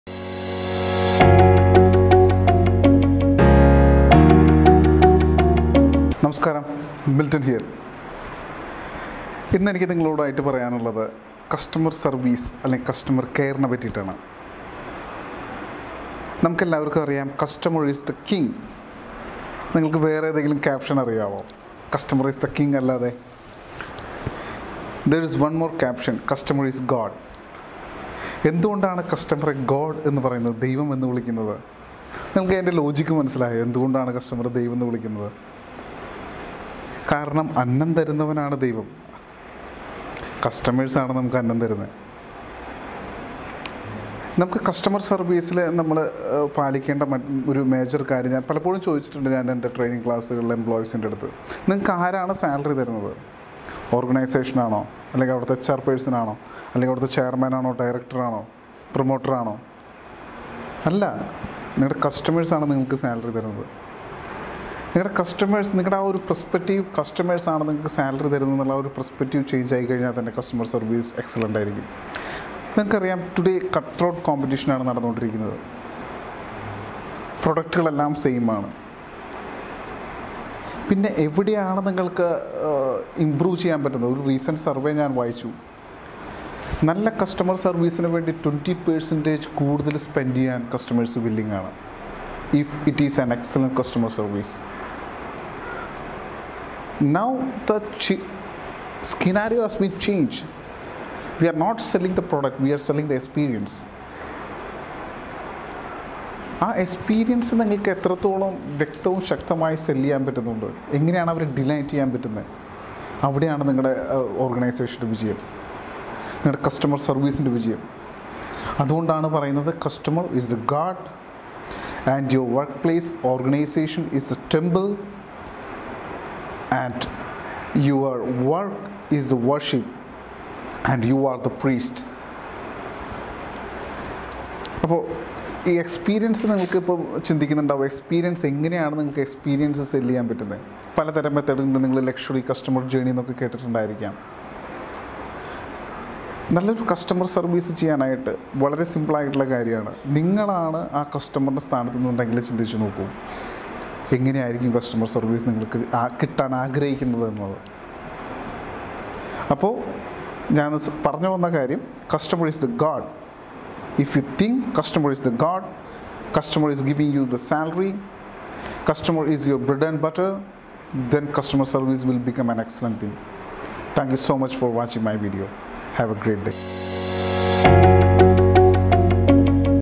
Why customer is the God | Milton M Joseph| Corporate Training
The logic behind the caption ' Customer is God ' .The value of customer service in business. We can buy the same products from many people, but why the customer should choose you. It is only because of the experience you are giving to your customers.